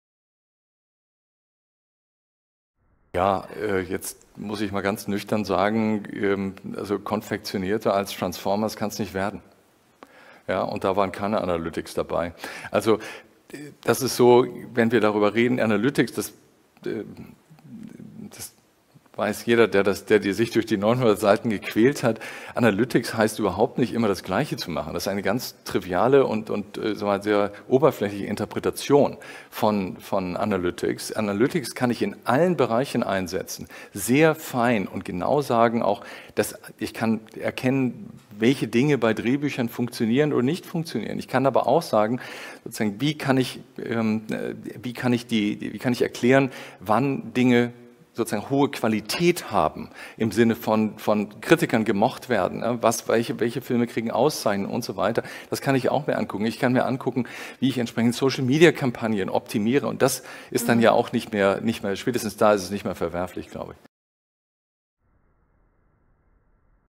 NRW Filmgespräch 9: Prof. Hennig-Thurau darüber, ob Entertainment Science Konfektionsware schafft
Streaming-Boom und Corona-Pandemie setzen die deutsche Filmwirtschaft zunehmend unter Druck: Kinofilme starten bei Netflix, die Zuschauer wandern von linearen zu nichtlinearen Anbietern, US-amerikanische Plattformen erobern den deutschen Markt.

Prof. Thorsten Hennig-Thurau, führender Wissenschaftler im Bereich Filmwirtschaft und Digitalisierung und Autor des Standardwerks "Entertainment Science", ist überzeugt, dass der digitale Umbruch eine grundlegende Neuorientierung in allen Bereichen des „Filmed Entertainment“ erfordert.

Im Gespräch mit der Journalistin Ute Soldierer gibt er einen empirischen Überblick über die aktuelle Situation und erörtert Handlungsoptionen für Einzelakteure und Marktpartner: Wie können deutsche Medienhäuser angesichts der Übermacht von Netflix & Co. ihre Relevanz bewahren? Was bedeutet die digitale Transformation für Produzenten? Hat das Kino eine Zukunft? Wie sollten Film- und Medienpolitik reagieren?

Das Gespräch fand am 27. Mai 2021 statt.

Veranstalter: Film- und Medienstiftung NRW